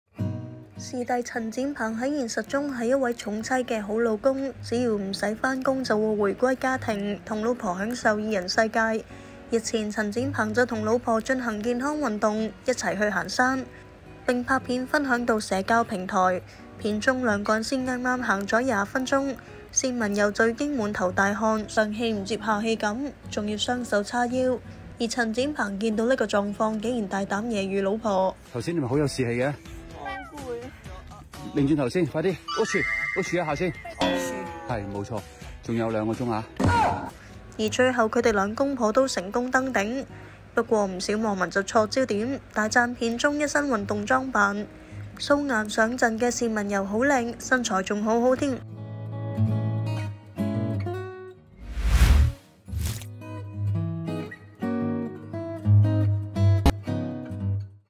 陳展鵬孖太太行山狂窒對方 單文柔獲激讚素顏太美了｜01娛樂｜陳展鵬｜單文柔｜香港人在北京｜無綫
視帝陳展鵬在過播完的無綫台慶劇《香港人在北京》中，演出大獲好評，陳展鵬現實中都是一位「寵妻」好老公，只要不用工作便會回歸家庭。日前，陳展鵬就與老婆進行健康運動，一齊去行山，並分享到社交平台。